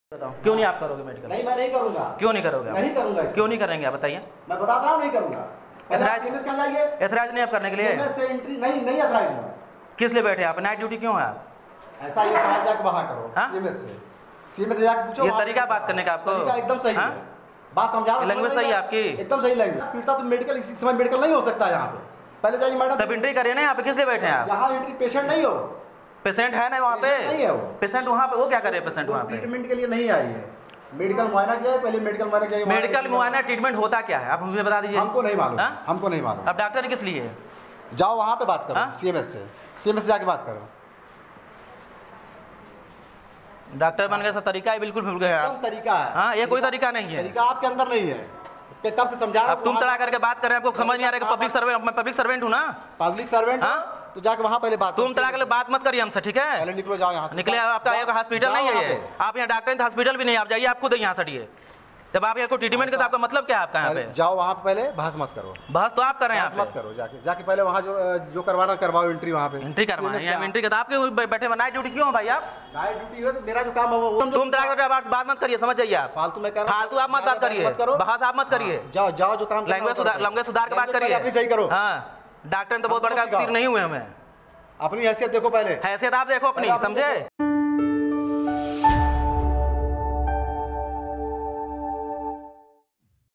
आजमगढ़: अस्पताल में तैनात डॉक्टर ने पुलिसकर्मी से की बदसलूकी, विडियो वायरल
आजमगढ़ जिला महिला अस्पताल में तैनात एक डॉक्टर द्वारा एक पुलिसकर्मी के साथ दुर्व्यवहार का विडियो सोशल मीडिया पर वायरल हो रहा है। निजामाबाद थाने का एक पुलिसकर्मी एक पीड़िता को मेडिकल करवाने के लिए महिला अस्पताल लेकर गया, जहां रात की ड्यूटी पर तैनात डॉक्टर असलम ने पीड़िता का मेडिकल करने से इनकार कर दिया। पुलिसकर्मी द्वारा मेडिकल के लिए ज़ोर दिए जाने पर डॉक्टर ने पुलिसकर्मी के साथ दुर्व्यवहार किया। सोशल मीडिया पर विडियो वायरल होने के बाद प्रशासनिक अमले में हड़कंप मच गया।